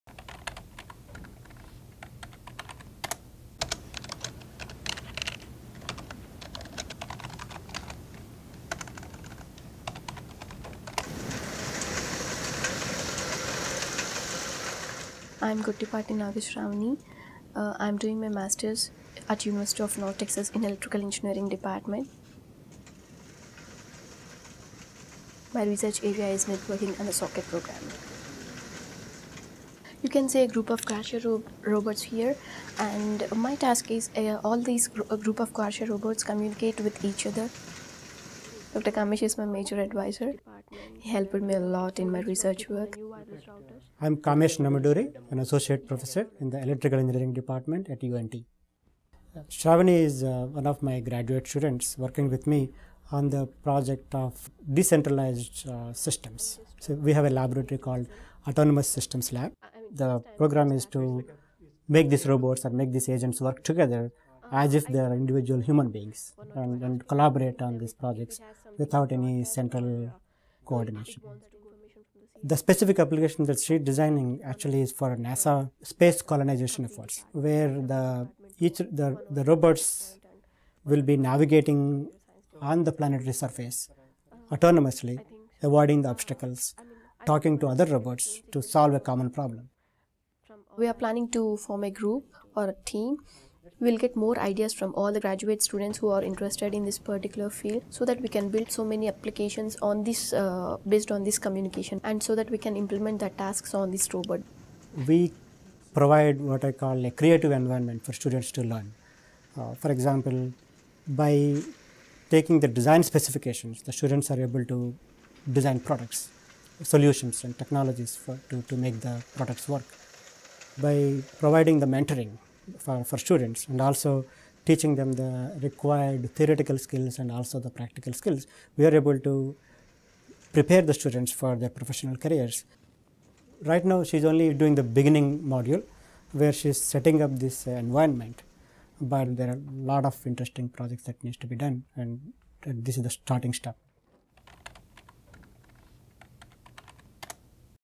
UNT Autonomous Systems Laboratory
University of North Texas graduate student Gottipati Naga Sravani and her faculty mentor Kamesh Namuduri, associate professor of electrical engineering, talk about their research in the autonomous systems laboratory at Discovery Park.